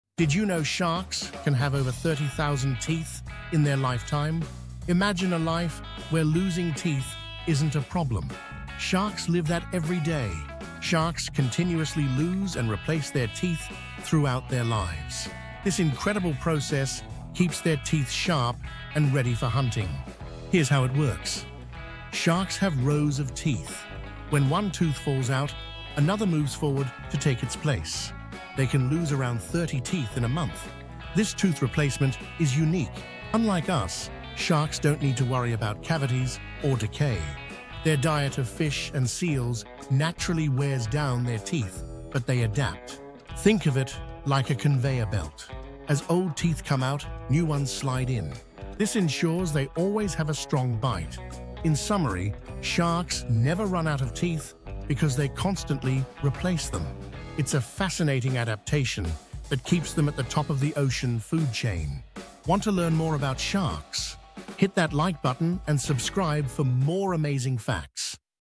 Why Sharks Never Run Out of Teeth 🦈🦷
Sharks continuously grow new teeth throughout their lives
Some species can replace thousands of teeth during their lifetime
Dive into the ultimate dental survival system with KnowledgeZap